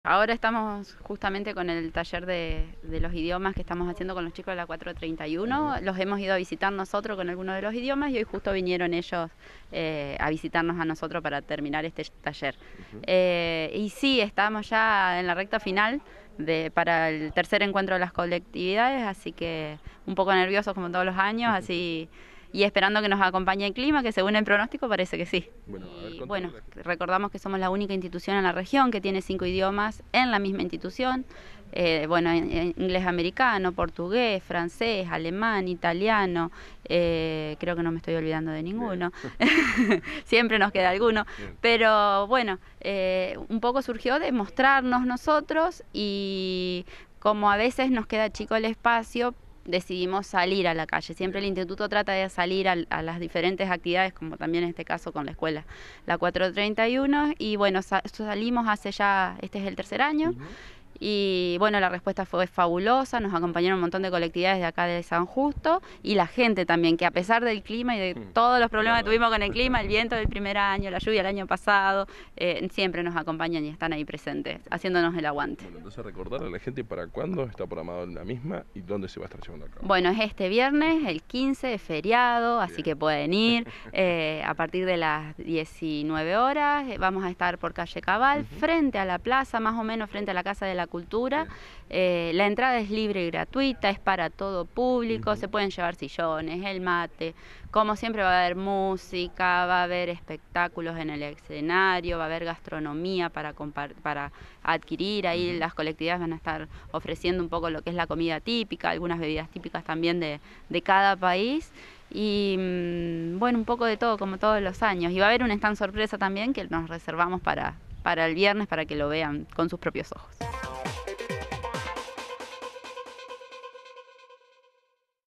Laura Pozzi
Directora del Instituto de Idiomas – Últimos detalles para la 3era. edicion del Encuentro de Colectividades